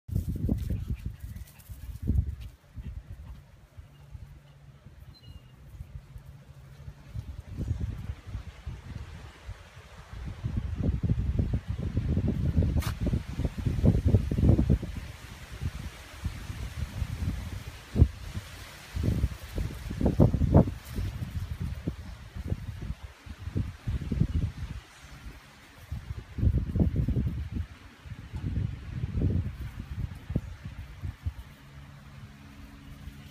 Video of adoptable pet named Maui
Maui is an adoptable pet with Canine Castaways, Inc. in Arcadia FL.